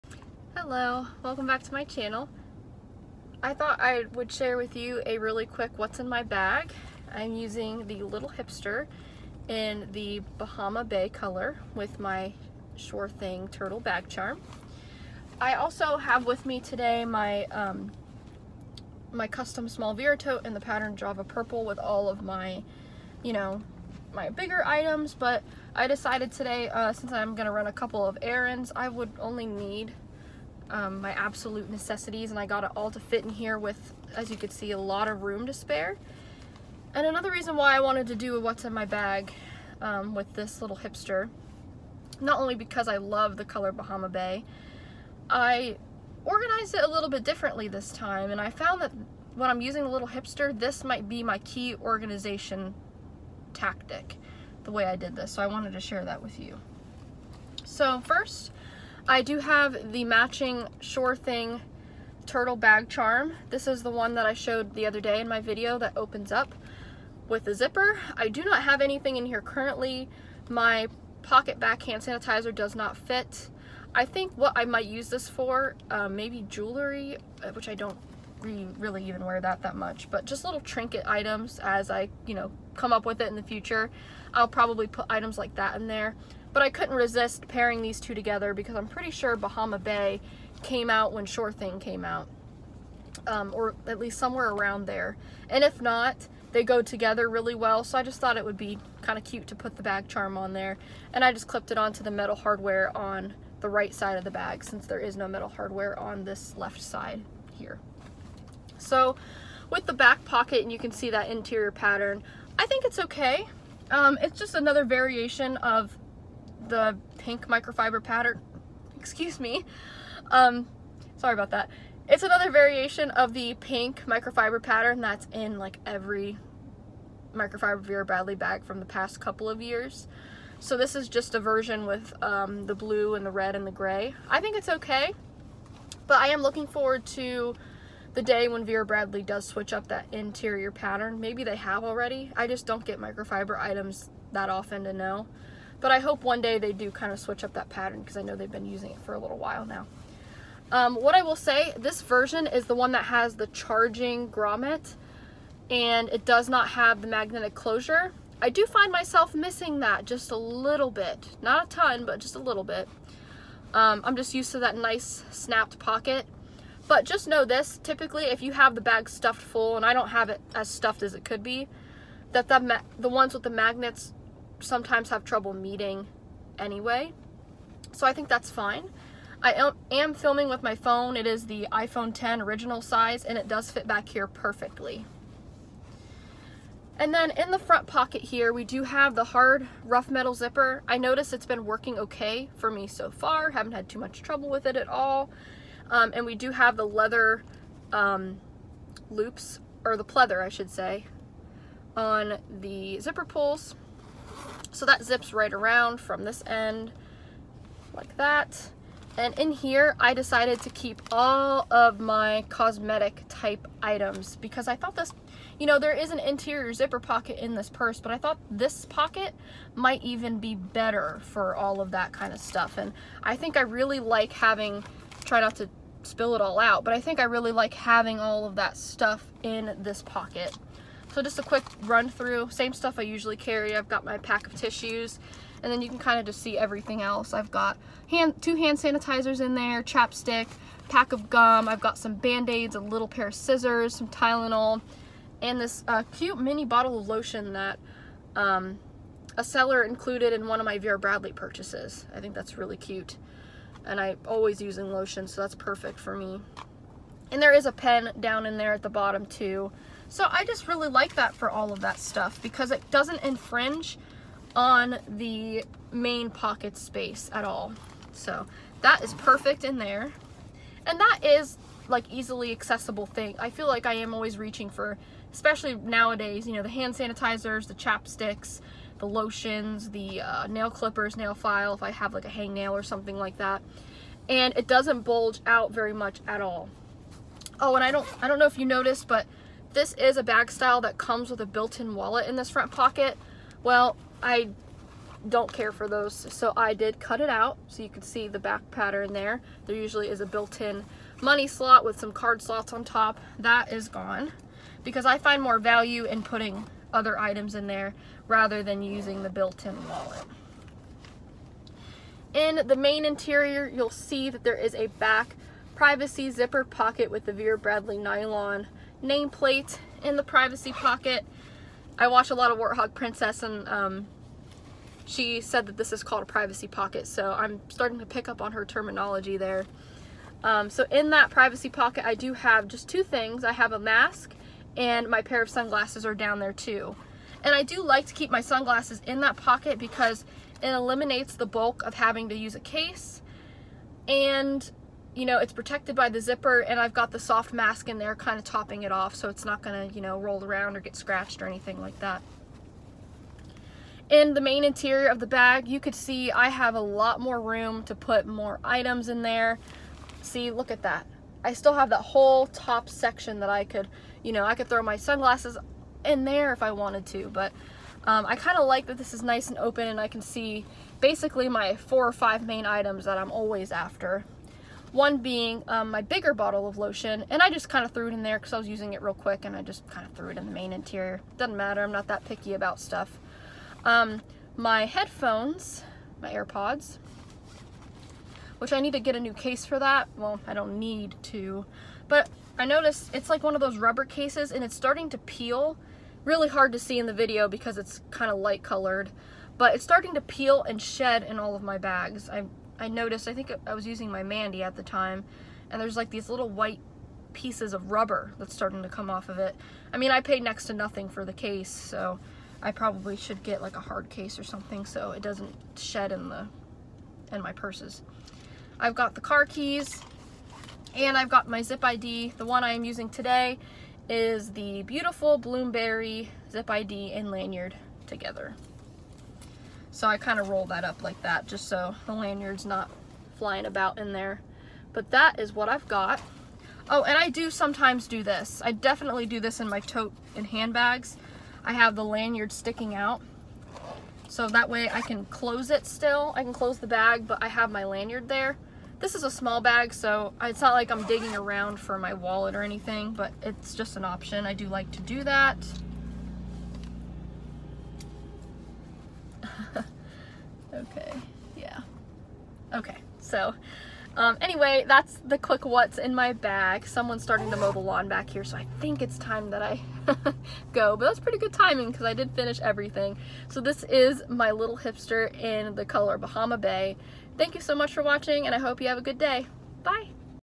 What’s in My Bag | Vera Bradley Bahama Bay Little Hipster
Today I’m using my Little Hipster in the beautiful summery Bahama Bay color. I show you how I packed it differently and why that works for me.

Items shown:

🌊 Bahama Bay Little Hipster
🐚 Shore Thing Turtle Bag Charm
🍓 Bloom Berry Zip ID Case & Lanyard